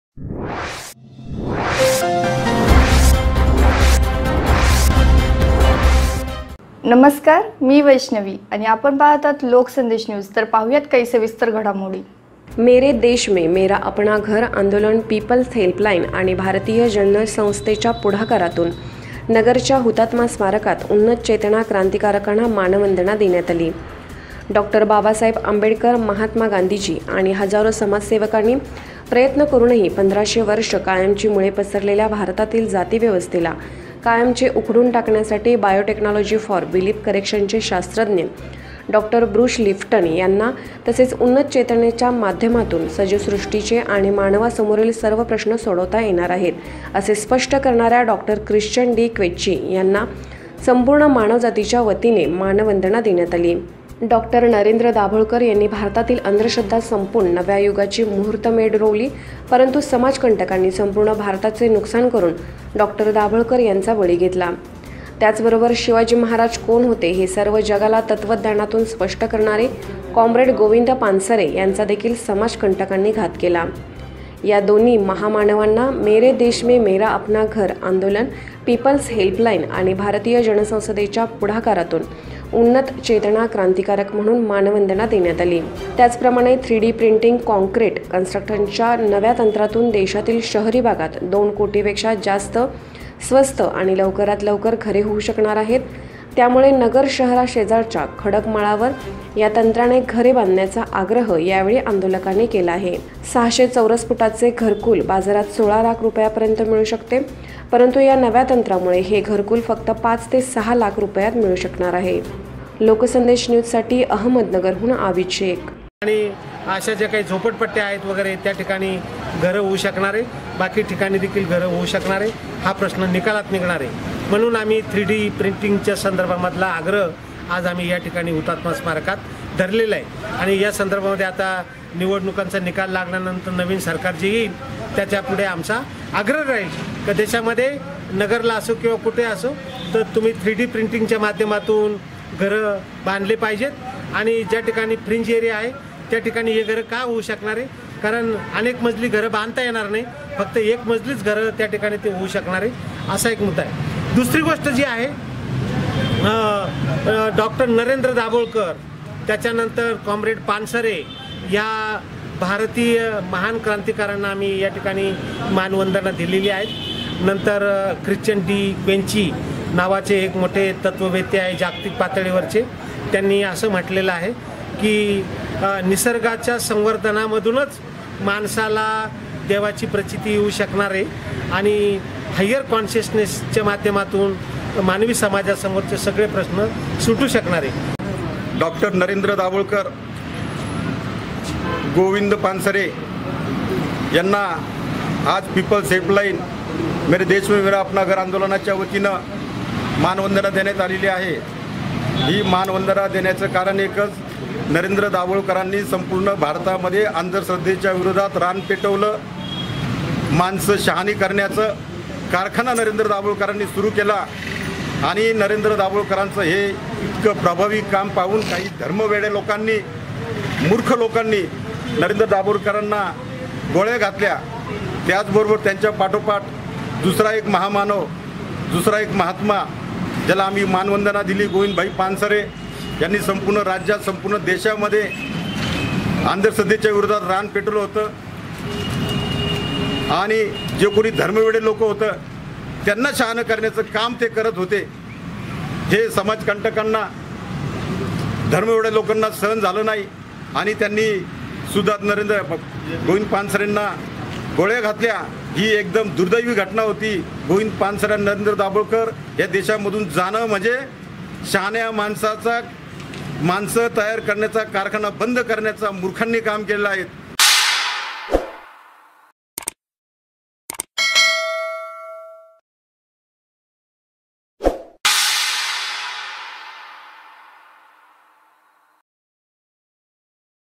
नगरच्या हुतात्मा स्मारकात उन्नत चेतना क्रांतिकारकांना मानवंदना
डॉ बाबासाहेब आंबेडकर महात्मा गांधीजी आणि हजारो समाजसेवकांनी प्रयत्न करूनही 1500 वर्ष कायमची मुळे पसरलेल्या भारतातील जाती व्यवस्थेला कायमचे उखडून टाकण्यासाठी बायोटेक्नॉलॉजी फ़ॉर बिलीफ करेक्शनचे शास्त्रज्ञ डॉ ब्रुश लिफ्टन  यांना तसेच उन्नत चेतनेचे माध्यमातूनच सजीवसृष्टीचे आणि मानवा समोरील सर्व प्रश्न सोडविता येणार आहेत असे स्पष्ट करणाऱ्या डॉ ख्रिश्चन डी क्वेंची यांना संपूर्ण मानव जातीच्या वतीने मानवंदना देण्यात आली डॉ नरेंद्र दाभोळकर यांनी भारतातील अंधश्रद्धा संपून नव्या युगाची मुहूर्तमेढ रोवली परंतु समाजकंटकांनी संपूर्ण भारताचे नुकसान करून डॉ दाभोळकर यांचा बळी घेतला त्याच बरोबर शिवाजी महाराज कोण होते हे सर्व जगाला तत्त्वज्ञानातून स्पष्ट करणारे कॉम्रेड गोविंद पानसरे यांचा देखील समाजकंटकांनी घात केला या दोन्ही महामानवांना मेरे देश मे मेरा अपना घर आंदोलन पीपल्स हेल्पलाइन  आणि भारतीय जन संसदेच्या  पुढाकारातून उन्नत चेतना  क्रांतिकारक म्हणून मानवंदना देण्यात आली त्याचप्रमाणे थ्री  डी प्रिंटिंग कॉंक्रीट कंट्रक्शनच्या नव्या तंत्रातून देशातील शहरी भागात दोन कोटी पेक्षा जास्त स्वस्त आणि लवकरात लवकर घरे होऊ शकणार आहेत त्यामुळे नगर शहरा शेजारच्या   खडक माळावर या तंत्राने घर बांधण्याचा आग्रह यावेळी आंदोलकांनी केला 600 चौरस फुटाचे घरकुल बाजारात 16 लाख रुपयांपर्यंत मिळू शकते परंतु या नव्या तंत्रामुळे हे घरकुल फक्त पाच ते सहा  लाख रुपयात मिळू शकणार आहे